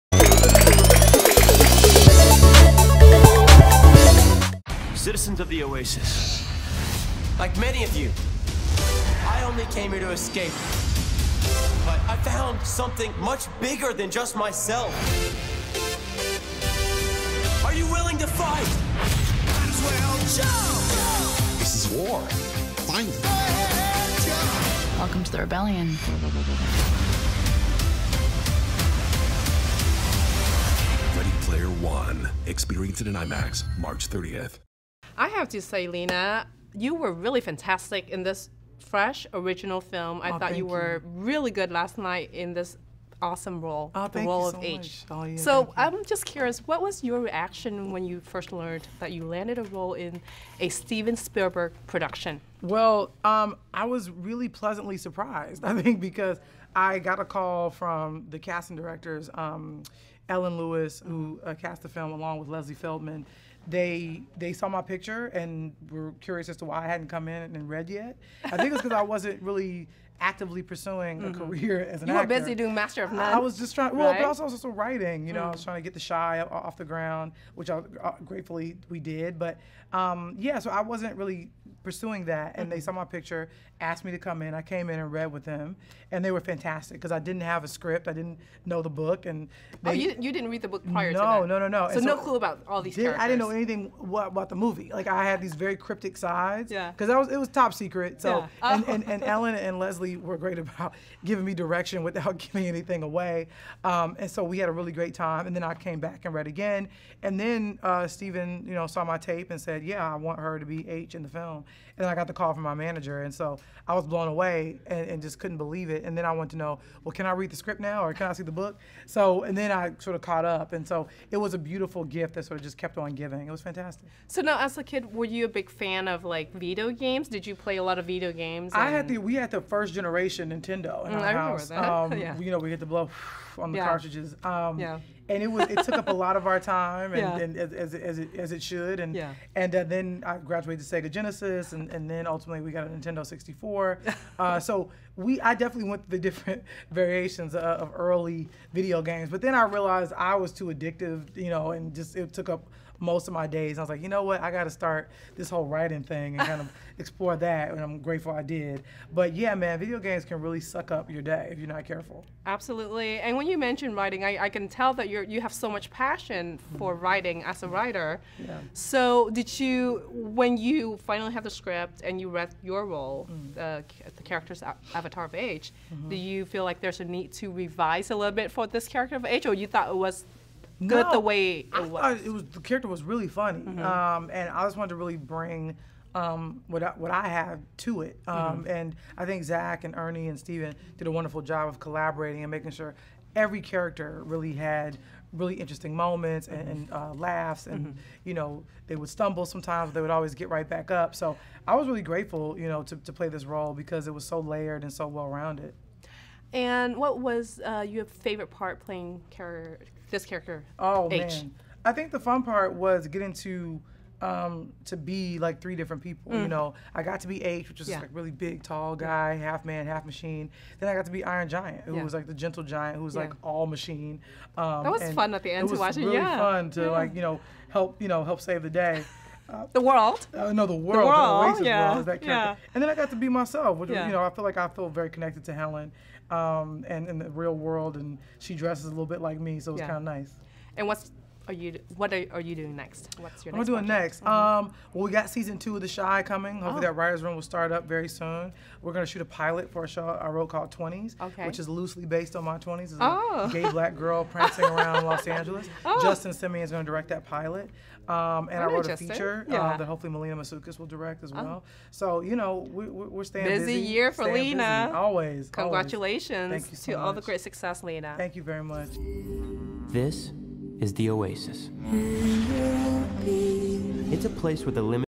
LENA WAITHE talks READY PLAYER ONE, Video Games, and More! (2018)
Follow HOLLYWOOD FIRST LOOK FEATURES